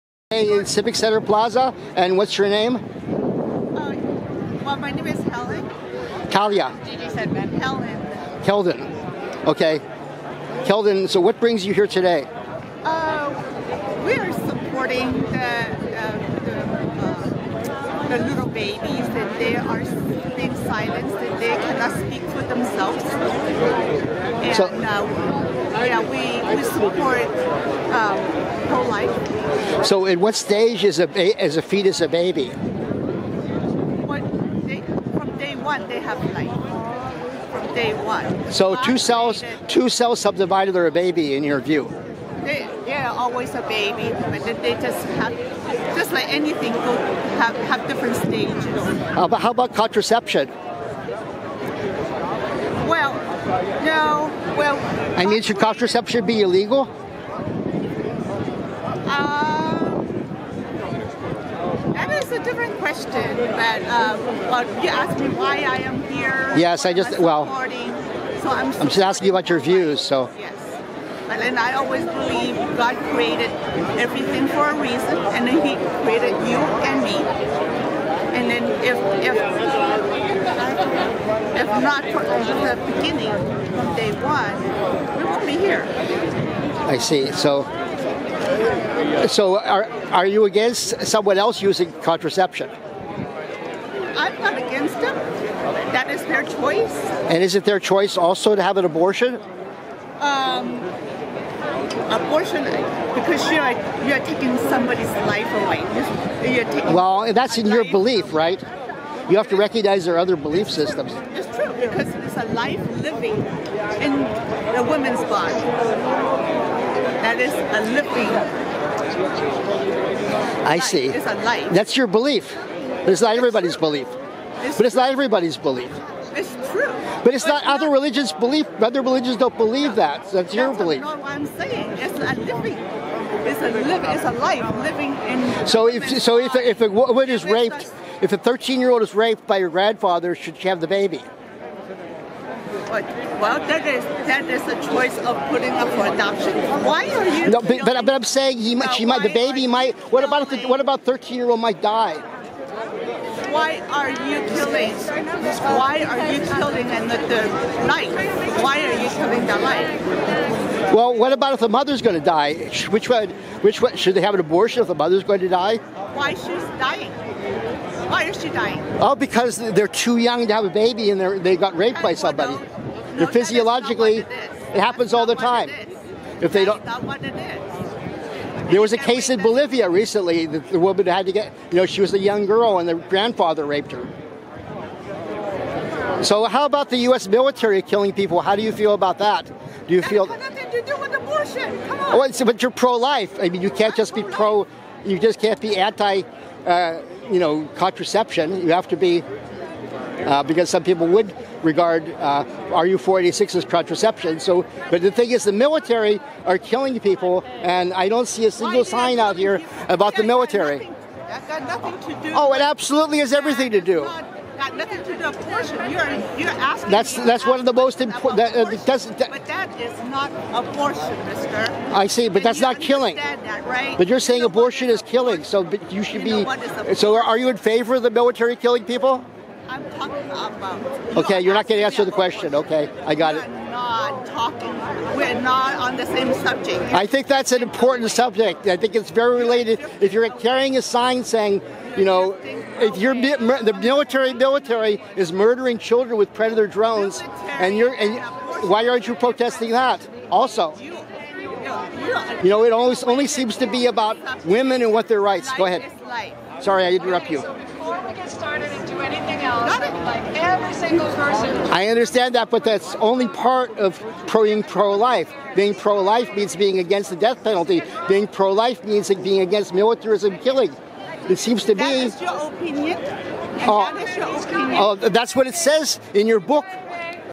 "The Eyes of God": Zealot speaks out on San Francisco anti abortion march
Unbelievable!

Civic Center Plaza, SF, CA, USA, Earth